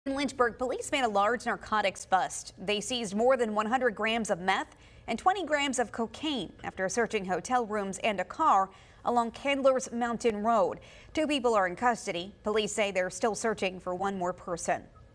Two facing several drug charges after Lynchburg PD allegedly seized over 100 grams of narcotics
The Lynchburg Police Department says two people have been charged with several drug offenses and they are searching for another after officers allegedly seized over 100 grams of narcotics on June 24.